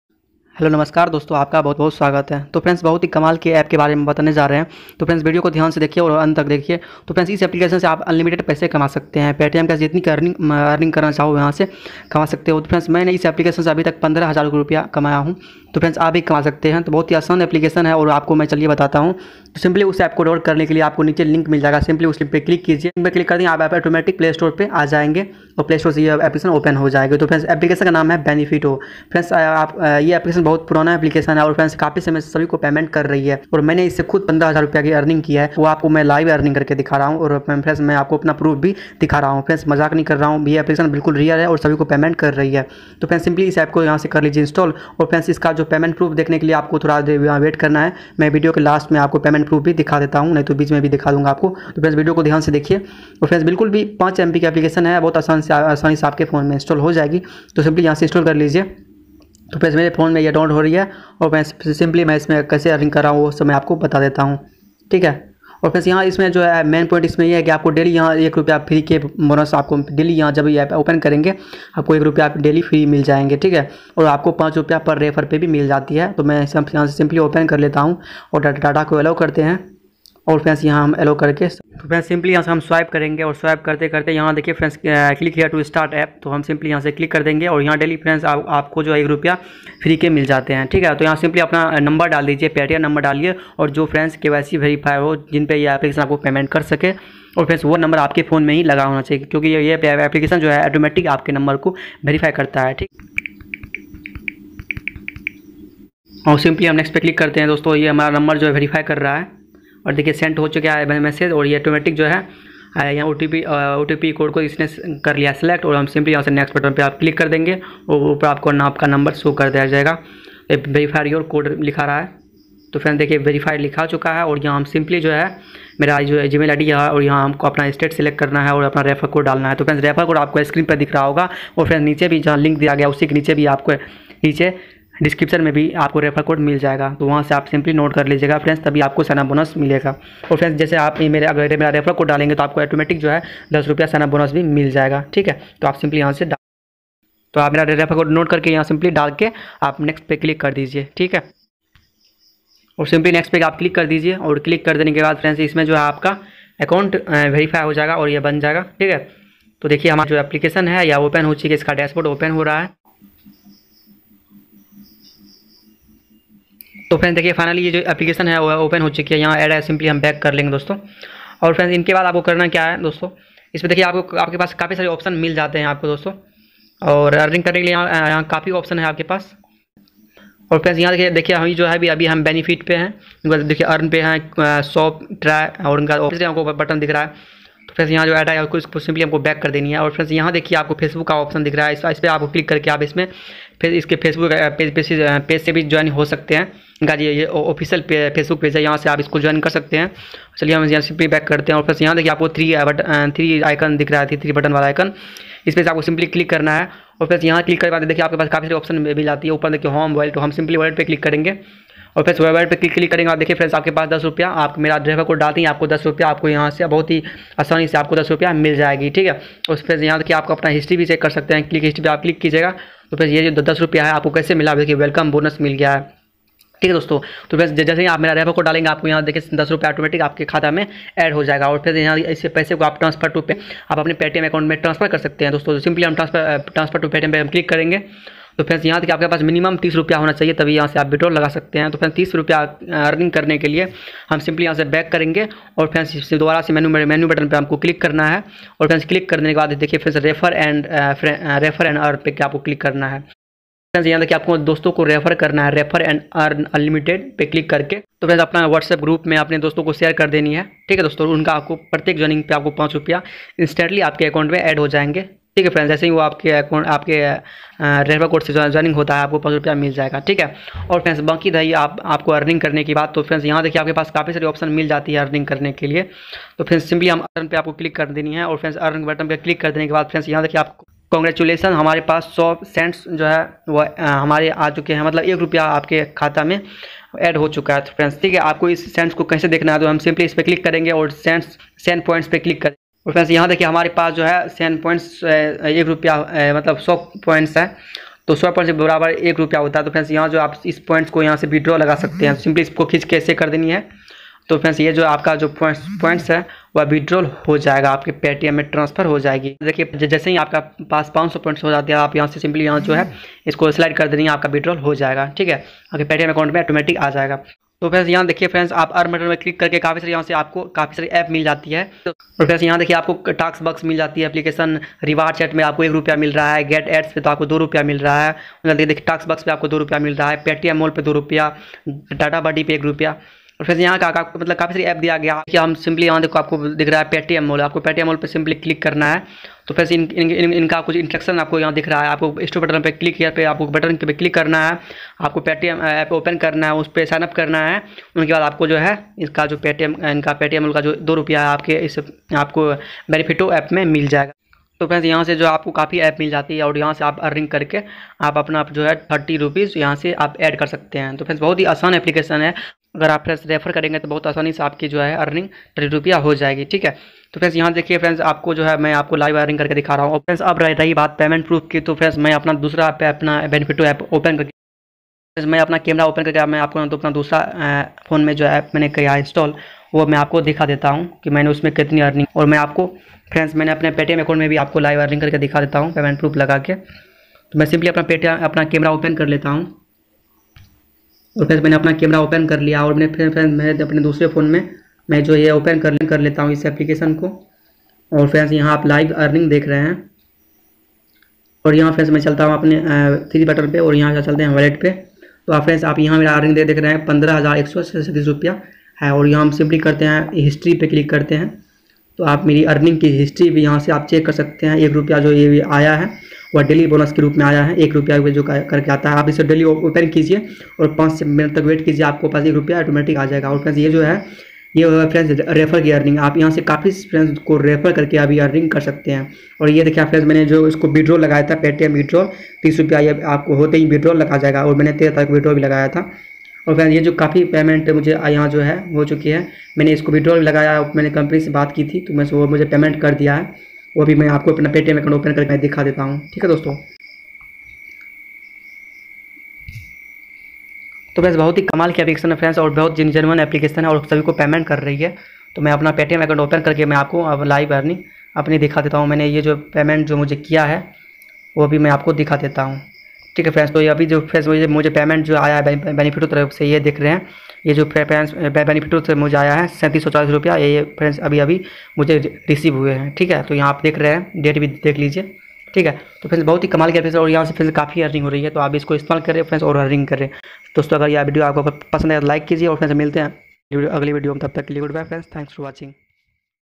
Earn ₹30/online यह App सबको दे रहा है free cash Direct Paytm में
Install &amp; Get Rs.30 Free Paytm Cash In Just 3 Minute Without Any Work. इस APP को install करते ही ₹30 रूपये FREE PAYTM CASH. Download Get & Rs30 free  Paytm Cash DAILY Earn. Old app benefito best genuine app.

Refer code :- UT5850448

Note: This is an informational video.

Hey! Namaskar Dosto Maine Aaj Aapko Is Video Mai Btaya Ki Kaise aap Unlimited Paytm Cash Ki Free Mai Eraning Kar Skte Ho Maine Jis Apk Ke Baare Mai Bataya Tha us Apk Ka Link aapko Niche de diya Gaya Hai.

Hello Fiends In This Video I Am Giving You A Tutorial On A New PayTM Earning App By Which You Can Easily Earn PayTM Cash Daily For Free. I hope You Will Enjoy The Video. 😘😘